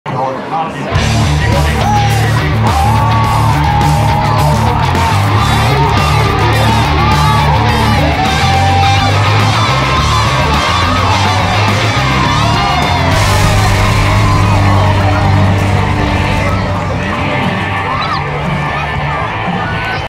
Bare Back 8 Seconds to Glory, Roy Rodeo
The Roy Pioneer Rodeo is where professional rodeo hands wrangle horses and other livestock in competition to test their skill and speed as cowboys and cowgirls. American-style professional rodeos generally comprise the following events: tie-down roping, team roping, steer wrestling, saddle bronc riding, bareback bronc riding, bull riding, and barrel racing.

Gear I use:
Insta360 One RS 1 inch
Sony A7III
DJI Air 2S
Selfie Stick